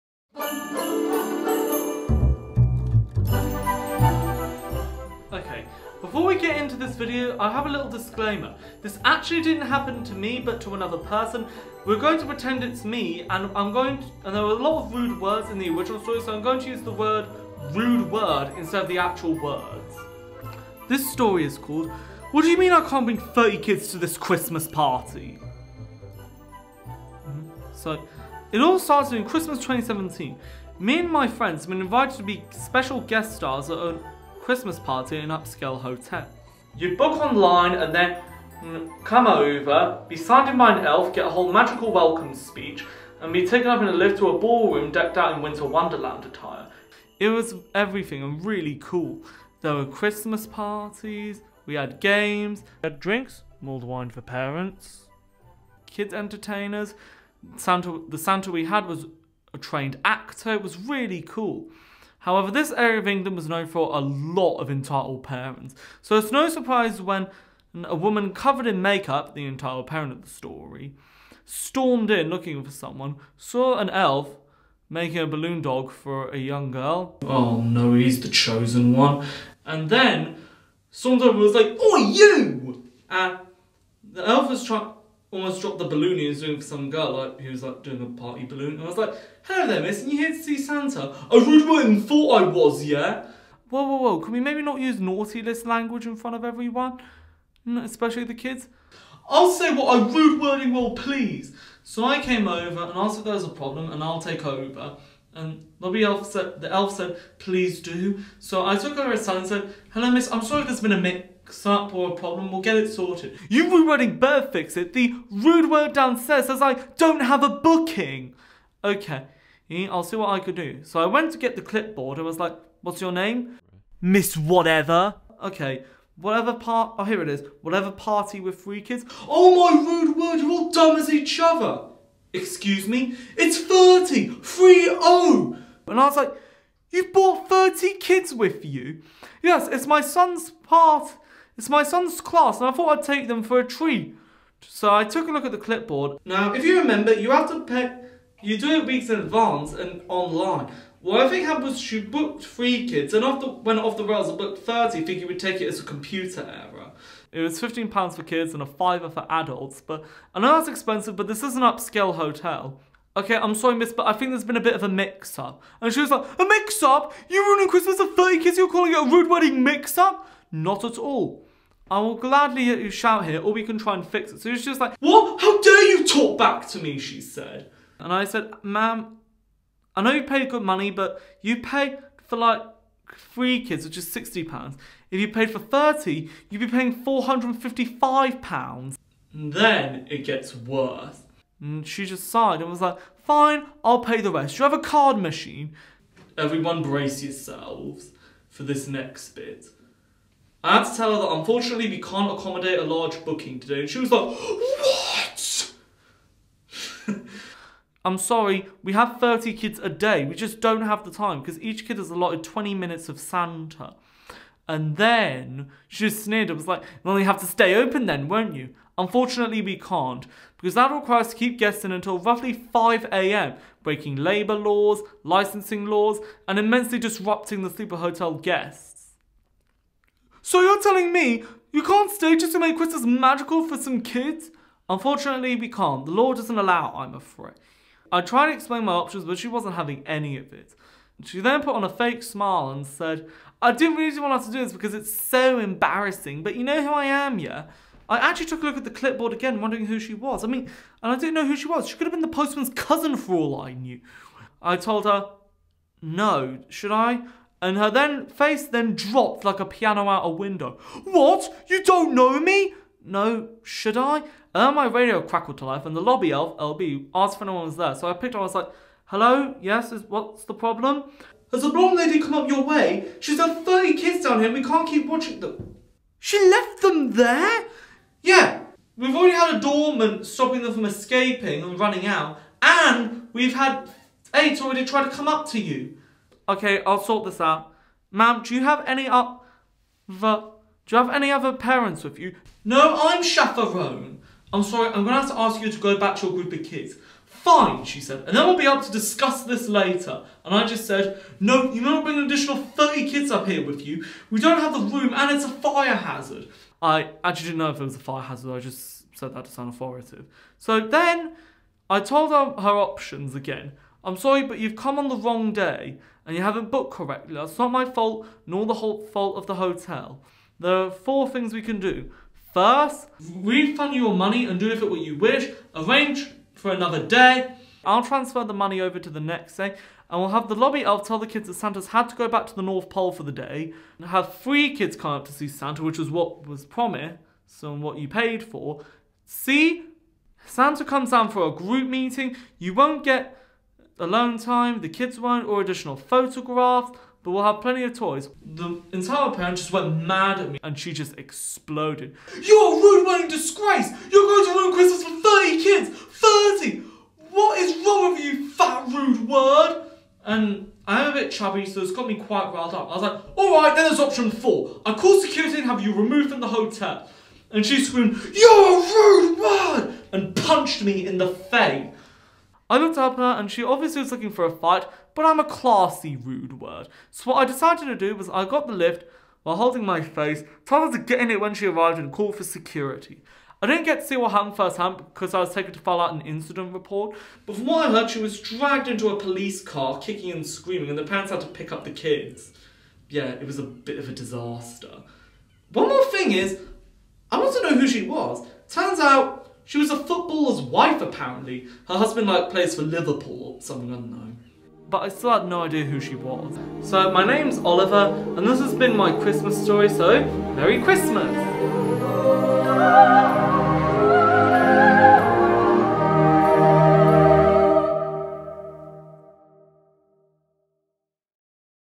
Oliver's FUNNY Christmas Story
This story is called
"WHAT DO YOU MEAN I CAN'T BRING
THIRTY KIDS TO THIS CHRISTMAS PARTY !?!?"

One day in session at Superstar Arts - Oliver stood up and told us this hilarious Christmas story..
It had us all rolling about laughing, he's got a real knack for bringing a story to life !
Oliver explained he had enjoyed the stories he had read on Reddit about 'entitled parents' causing trouble .. and told his interpretation of one he found particularly enjoyable.

You can support our charity by making a purchase on our online shop,
all products are made by our learning disabled artists